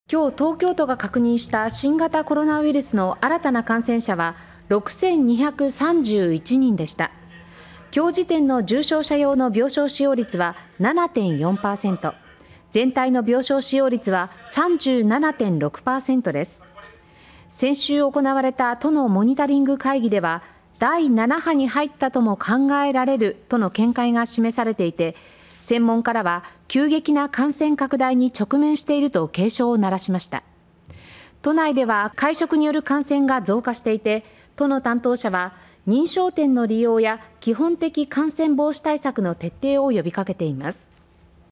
【速報】東京の新規感染6231人 (2022年7月11日)
11日に東京都が確認した新型コロナウイルスの新たな感染者は6231人でした。

　11日時点の重症者用の病床使用率は7.4％、全体の病床使用率は37.6％です。

　先週に行われた都のモニタリング会議では「第7波に入ったとも考えられる」との見解が示されていて、専門家らは急激な感染拡大に直面していると警鐘を鳴らしました。

　都内では会食による感染が増加していて、都の担当者は認証店の利用や基本的感染防止対策の徹底を呼び掛けています。